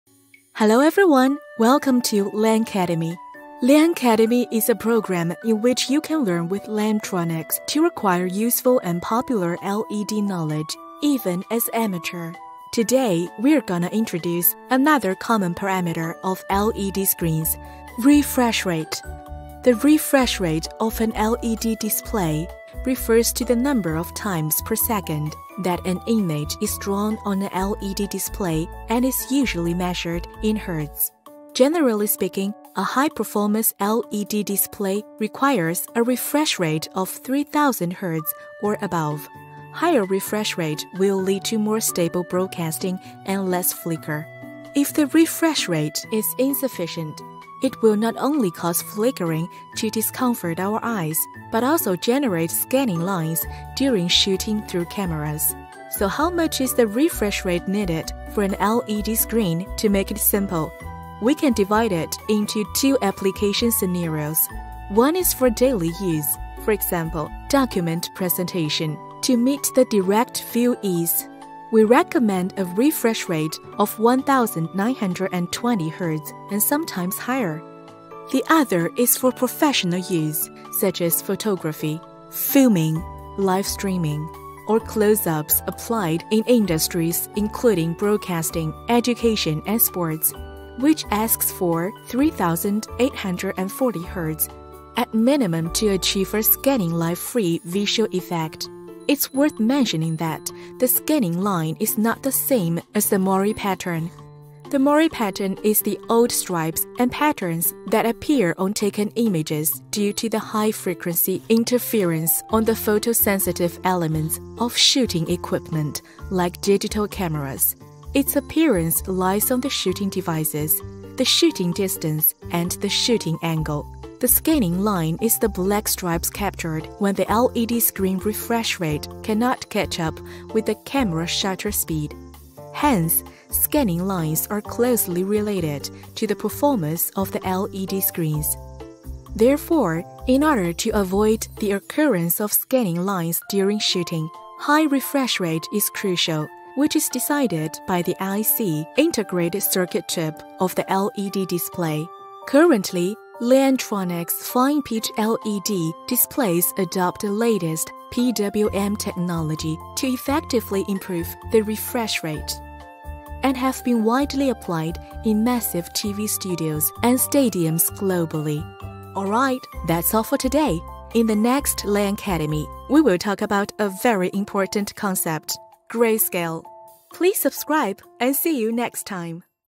Liancademy: How to choose the appropriate refresh rate for an LED screen?
Liancademy" is a program in which you can learn with LianTronics to acquire useful and popular LED knowledge even as an amateur.

How much refreshrate does a high-performance LED screen ask for?
What refresh rate configurations do we often use?
What’s the difference between scanningline and moirépattern?
What determines the performance of the refresh rate of an LED wall?

One video, you can know all about refresh rate!